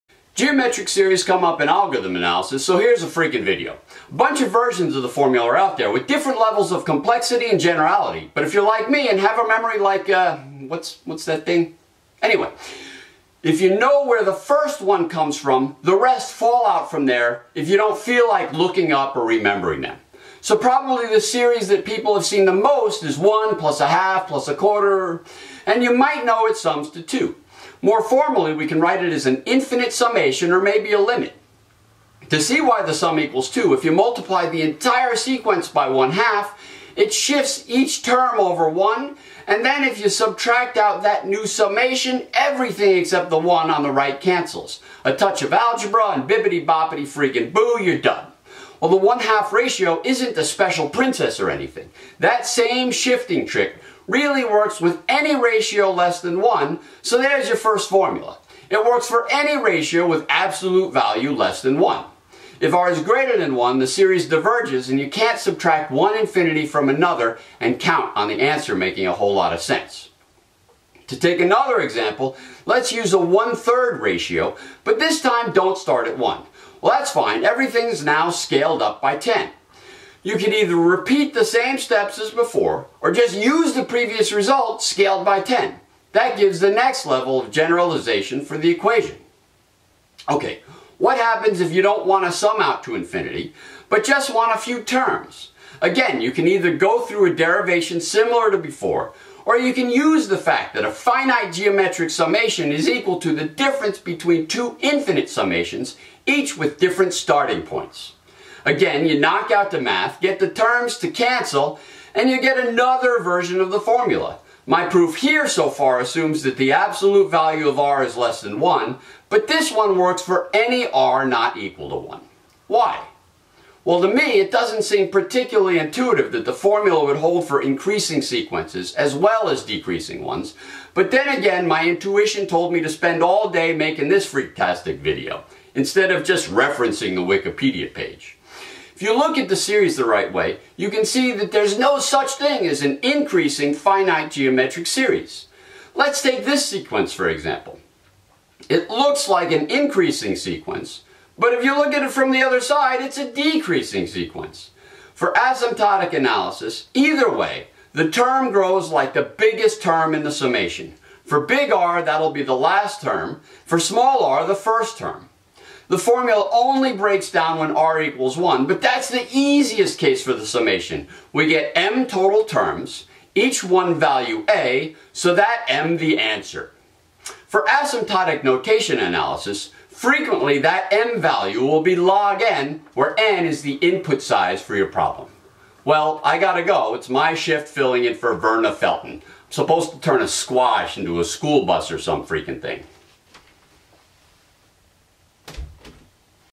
Geometric Series for Algorithms Analysis
Geometric series, presented at a level appropriate for basic algorithm analysis.

Table of Contents:
00:00 - Introduction
00:20 - Decreasing Case
02:05 - Increasing Case
02:45 - Flat Case